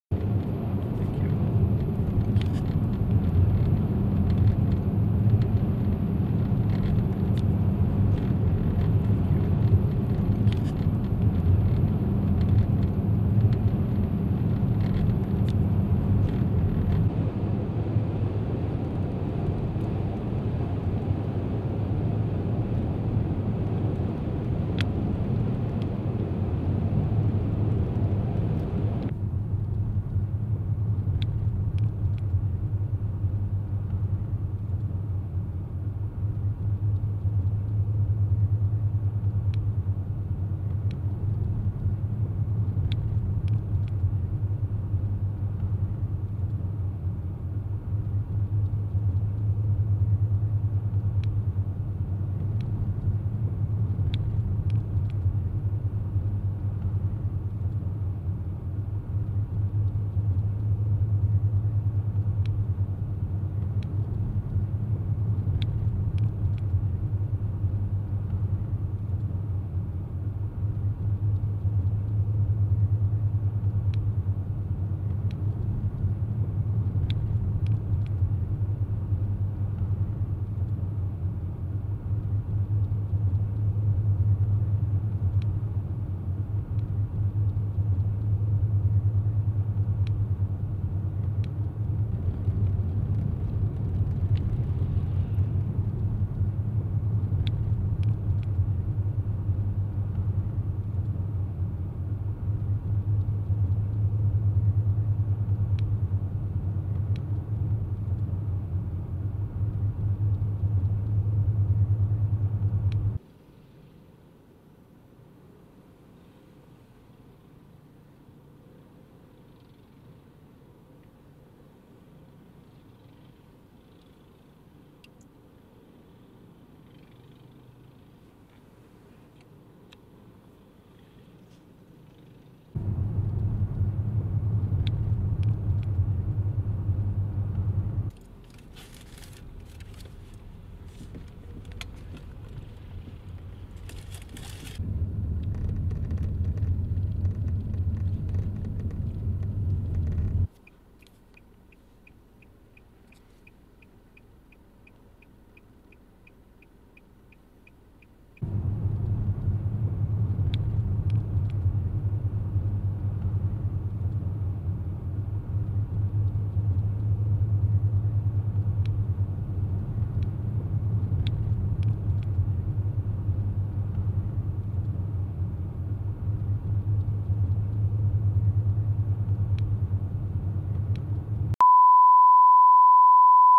Blizzard Closes Interstate 80 and Interstate 25 in Cheyenne, WY
April Blizzard conditions closed I-80 west and I-25 north of Cheyenne, WY overnight.

Shot Description

1. Snowplow clears I-25 northbound in Wyoming
2. Info road sign for I-25 Interstate Closure -Return to Cheyenne-
3. Flashing lights for Closed I-80 West
4. Closure Signs for I-80 West
5. I-80 Closed
6. Police enforce I-80 Westbound ramp
7. Road info sign
8. Cheyenne, WY town sign
9. Front end loader trying to free car
10. Car stuck in Cheyenne, WY
11. Gang of Snow blowers
12. 4-wheeler snow plow
13. Snow blower
14. Old Steam Train in Snow
15. Bank temperature sign of 20 degrees
16. Road to I-25 north of Cheyenne
17. Traffic on I-25